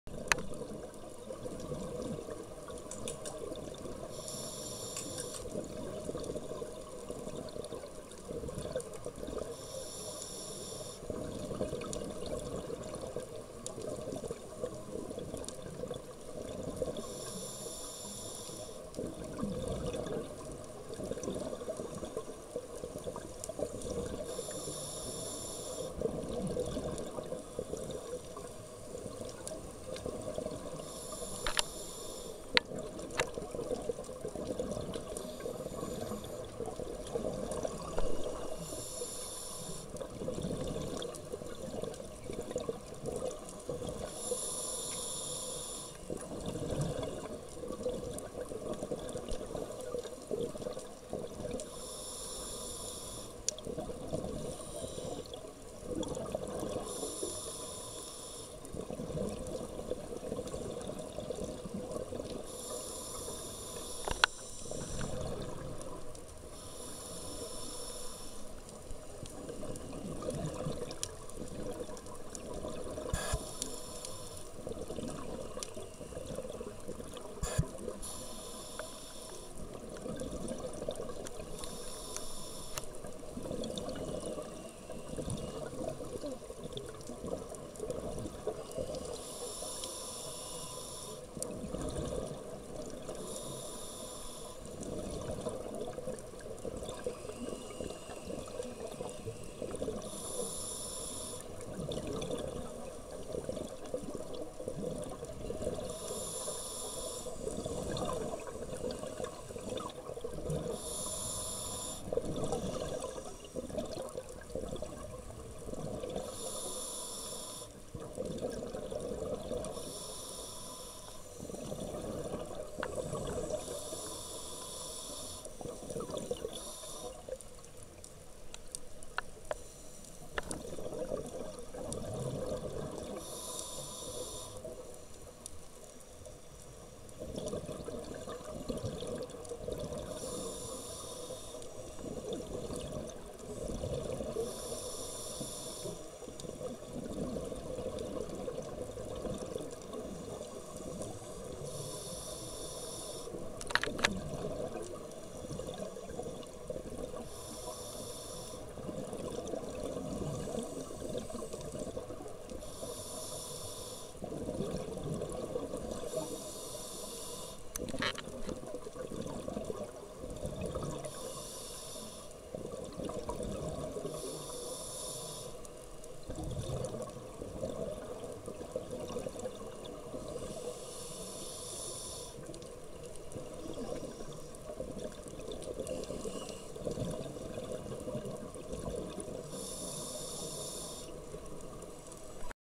Whale Shark @ Redang Island - Sept 2011
Our unforgettable encounter with a magical whale shark during our dive trip to Redang Sept 2011 :)